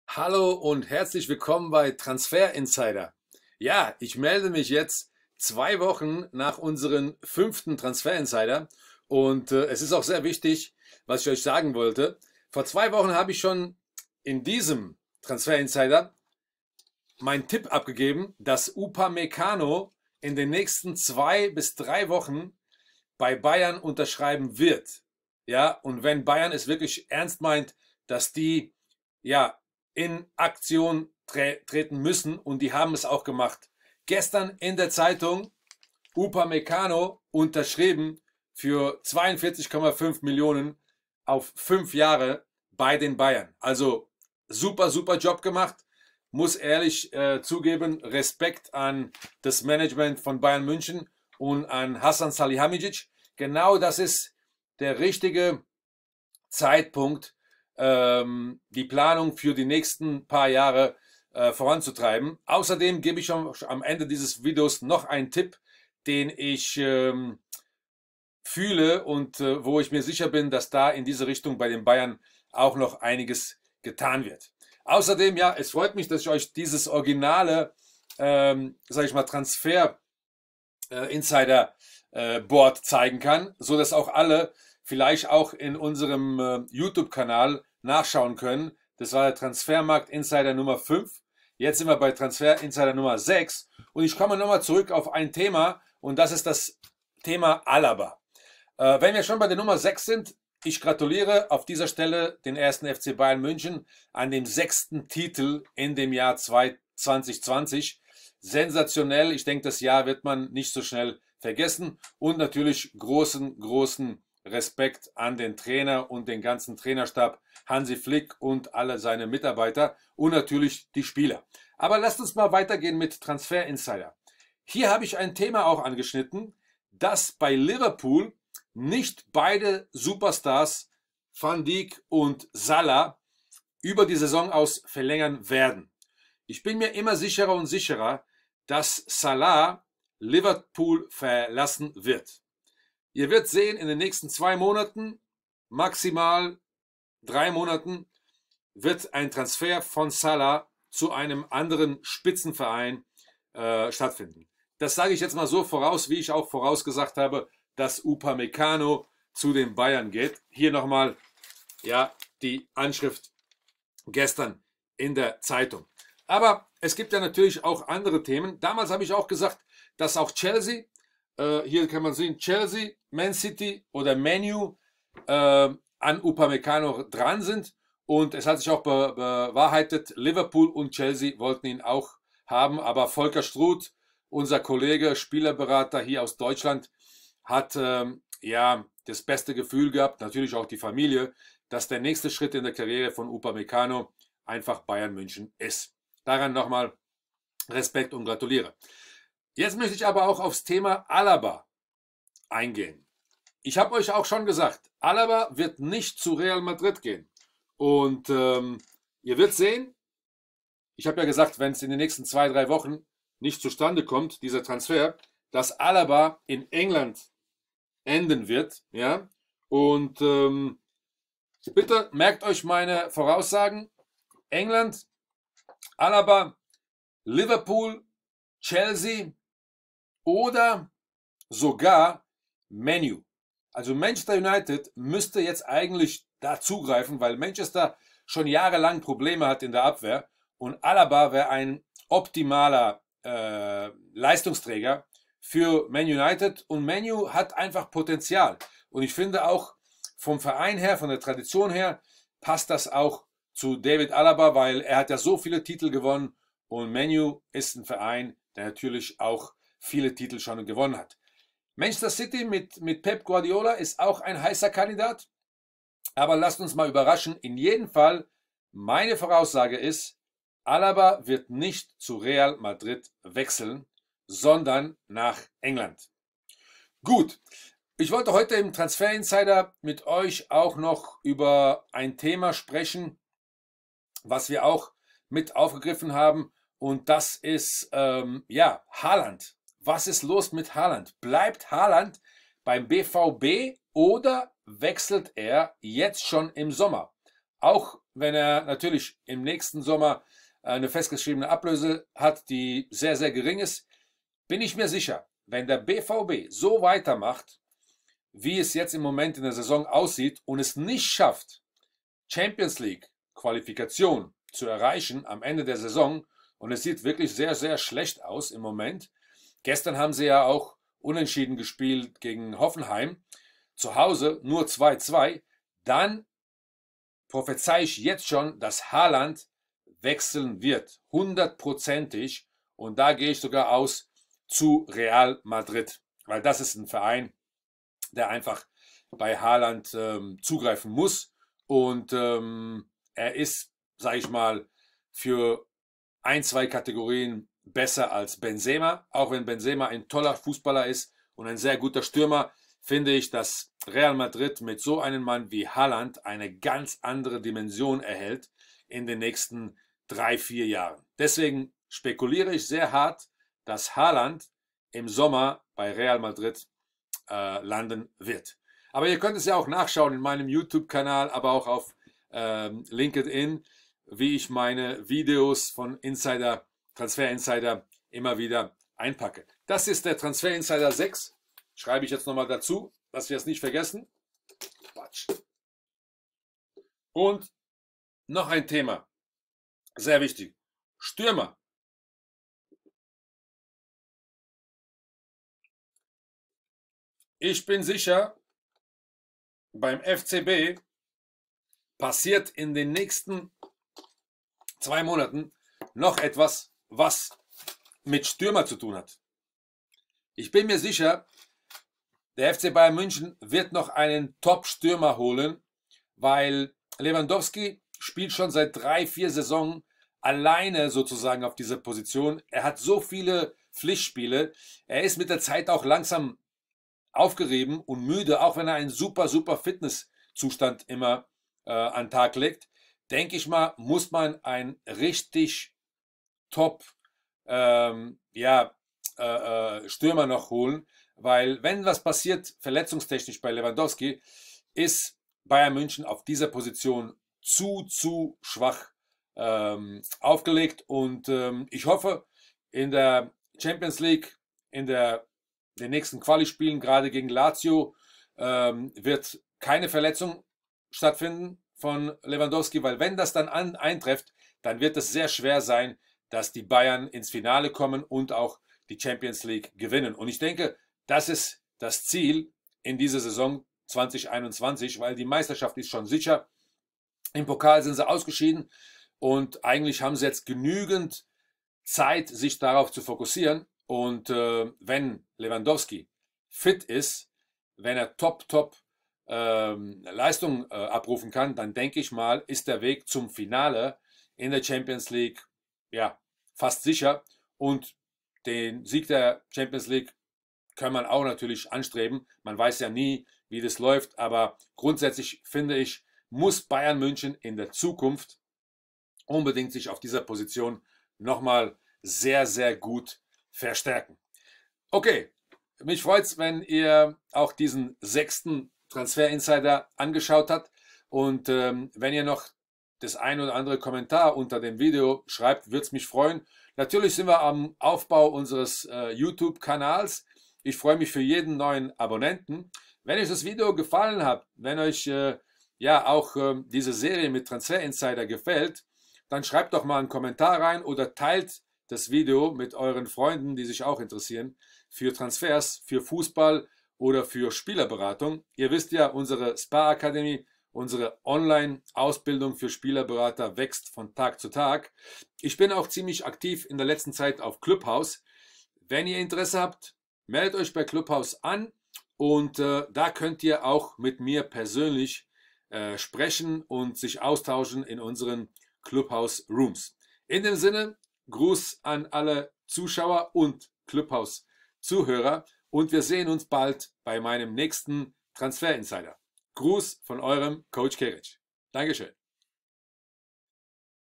6Transfer-Insider/Van DIJK/Upamecano bei FCB-Unterschrieben/ALABA nach England
In diesem Video spreche ich über die Zukunft von Van Dijk und es bestätigt sich meine Prognose das Bayern München in den nächsten 2 Wochen den Deal mit Upamecano klar macht. Ich prophezeie hier auch das David Alaba nach England wechselt im Sommer.